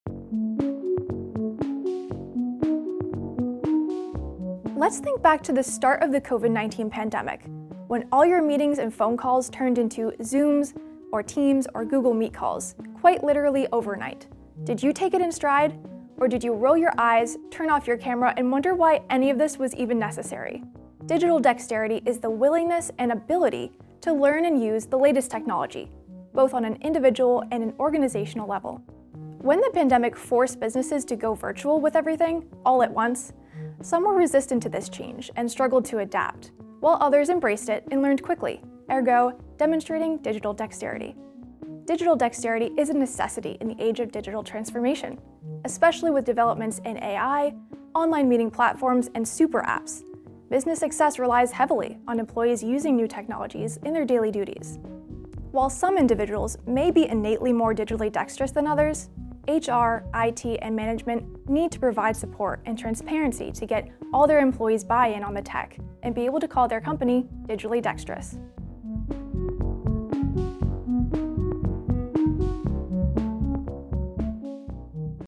What is Digital Dexterity? (in about a minute)
How tech savvy are you? Or, should we say, digitally dexterous? Digital dexterity is key to efficient workforces in the age of digital transformation, Web3, and virtual everything. So, HR, management, and IT need to arm their staff with the resources and support to become digitally dexterous.